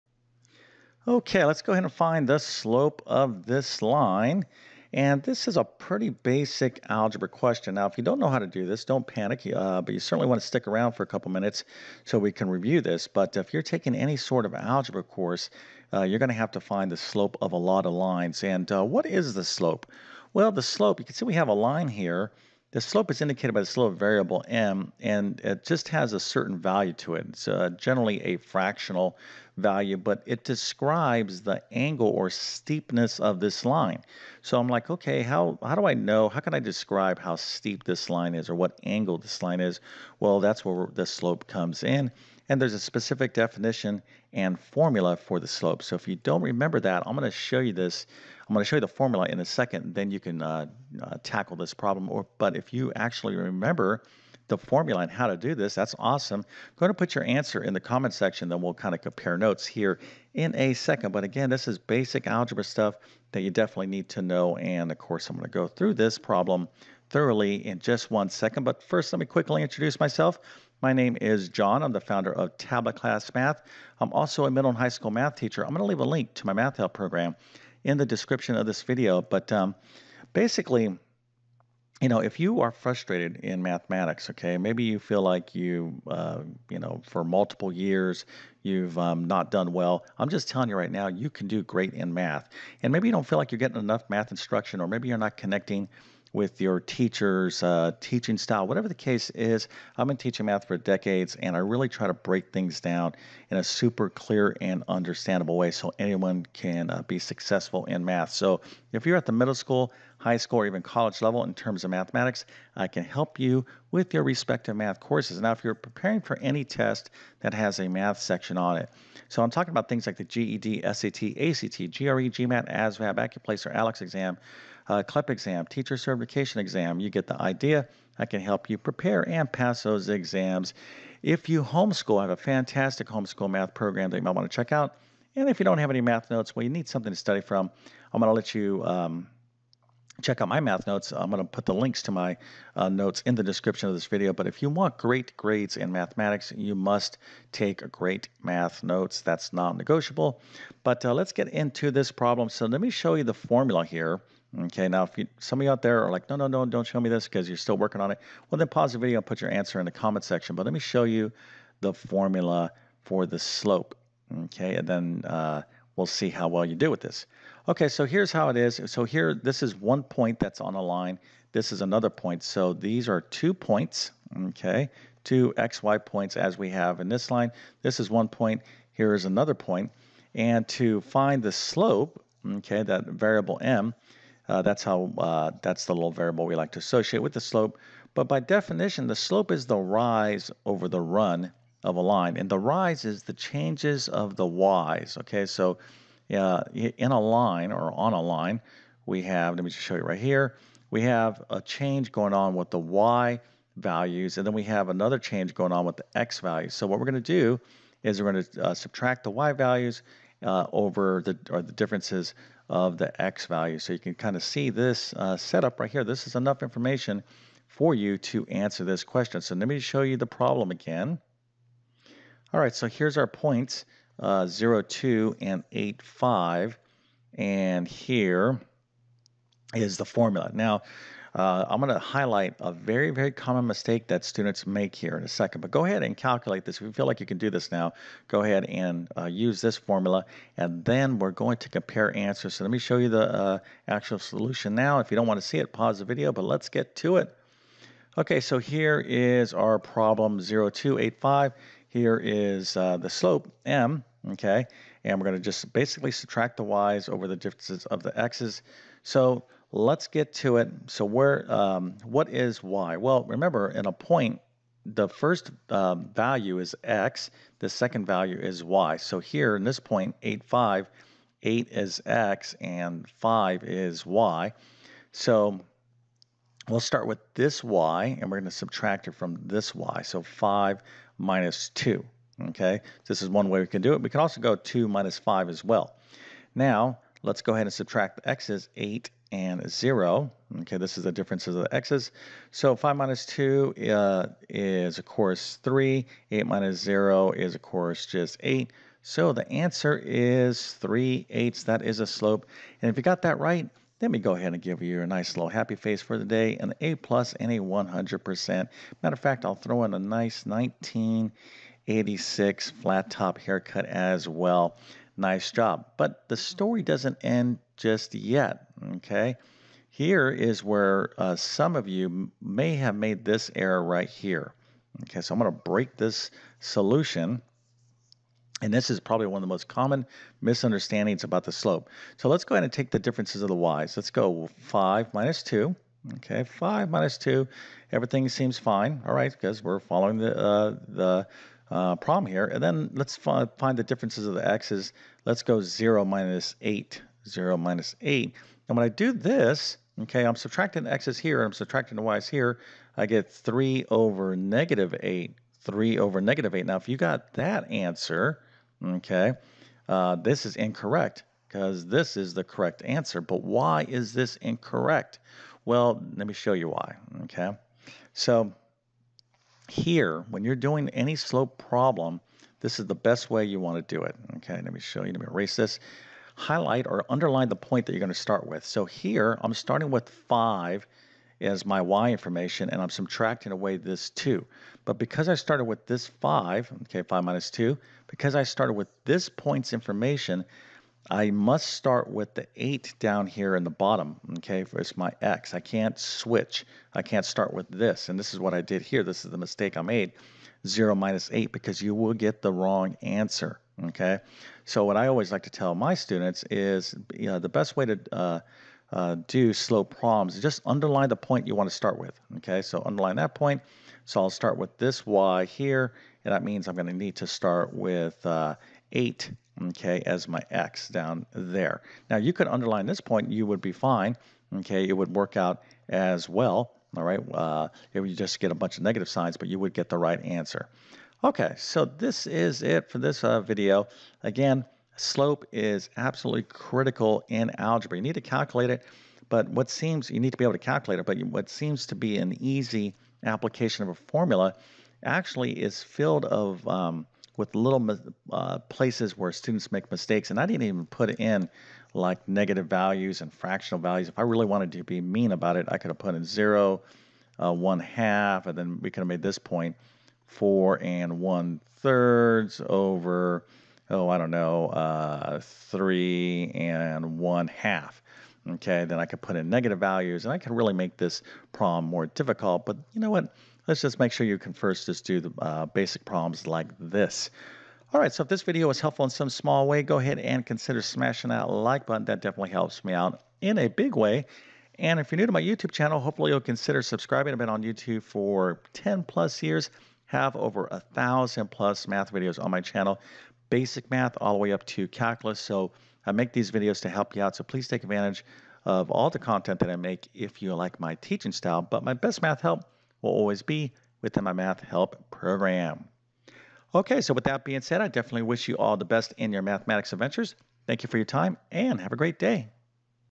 Find the Slope – Basic Algebra Skill
How to find the slope of a line.  For more math help to include math lessons, practice problems and math tutorials check out my full math help program